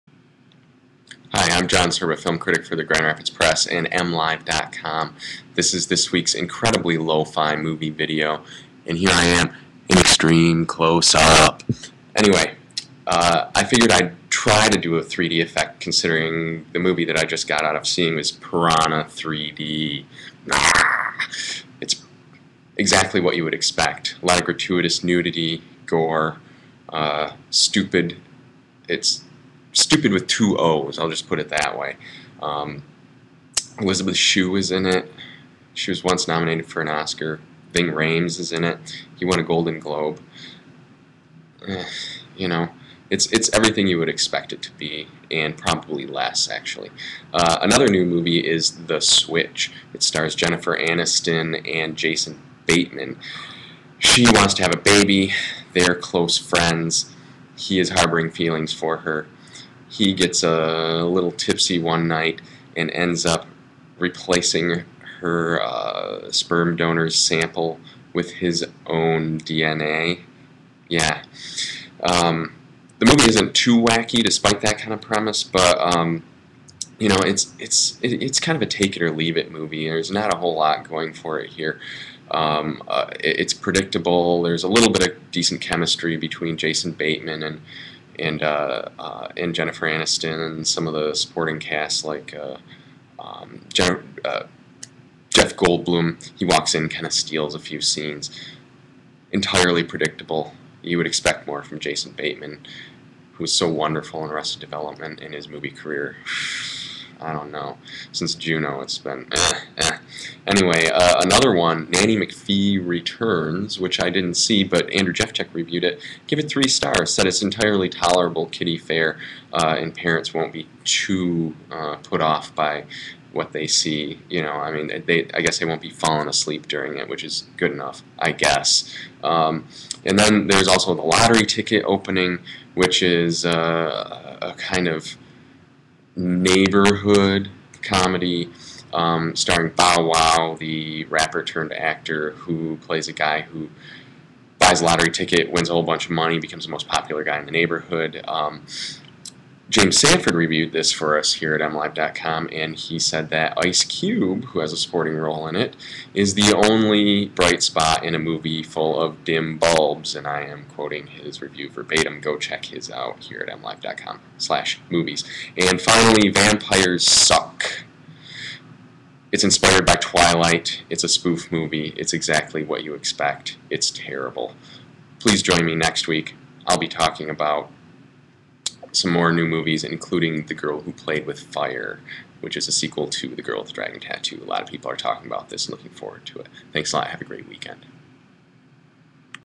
This week's movies in 3 mintues: "The Switch" and "Piranha 3D"
MLive film critic John Serba